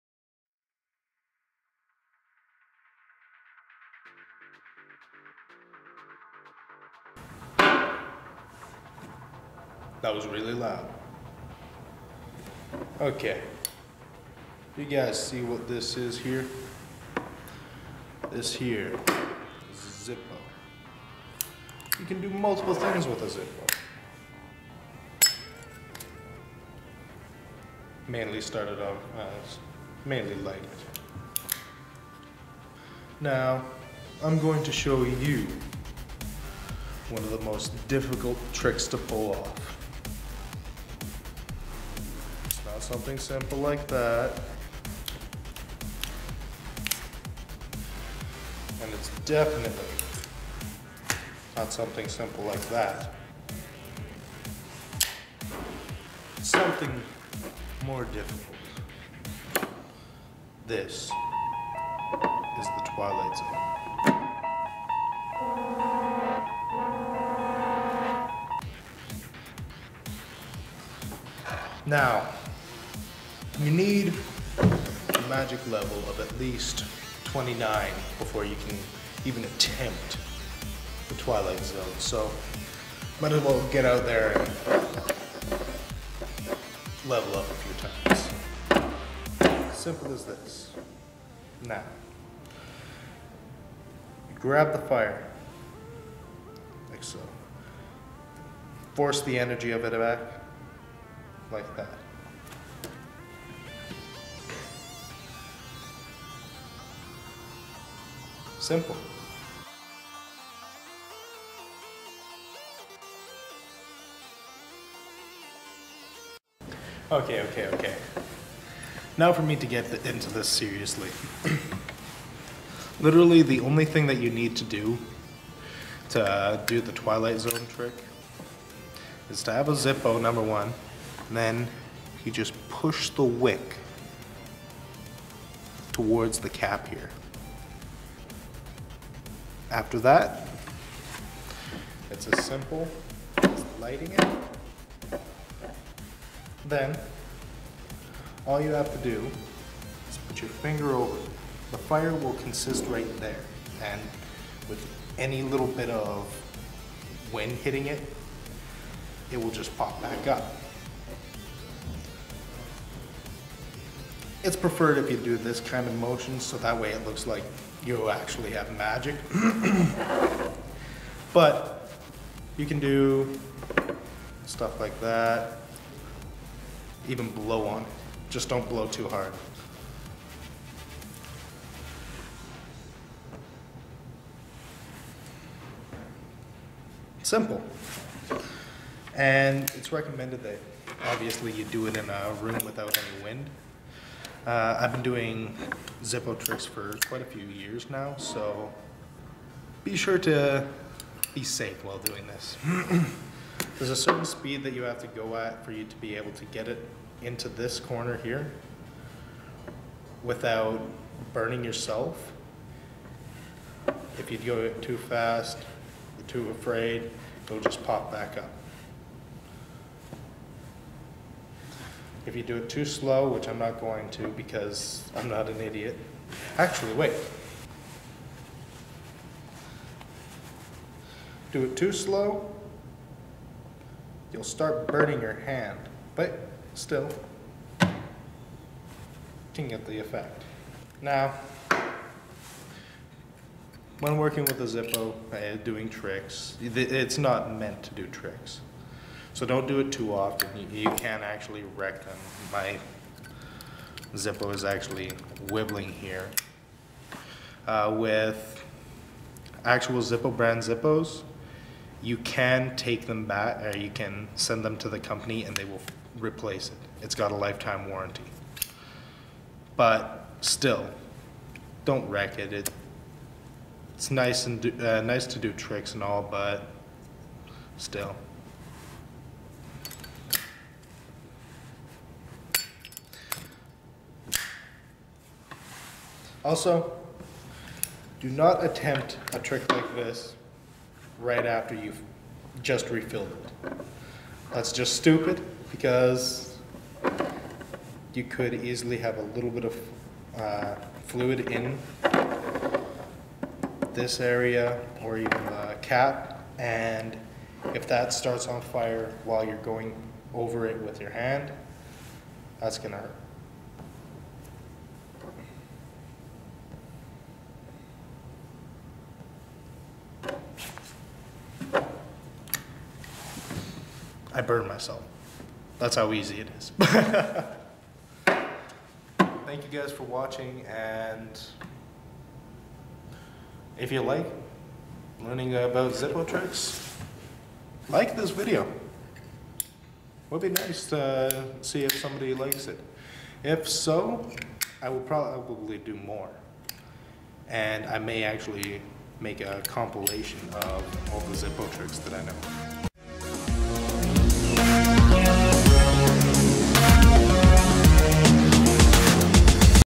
The Twilight Zone (Magic)
Within this video I show the video off the zippo trick called the twilight zone. please enjoy and check out the other videos on my channel